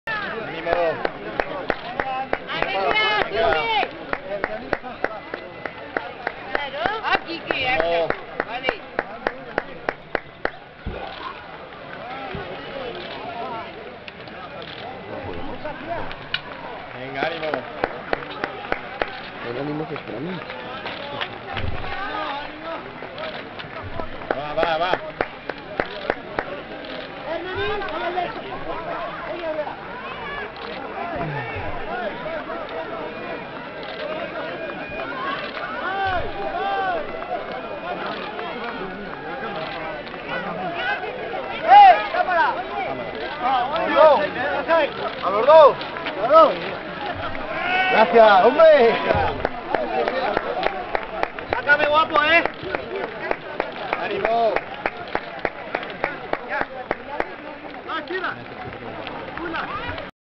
Gamero en el Marie Blanque
Gamero coronando el Marie Blanque en la Quebrantahuesos 2009 (00:14 a 00:24)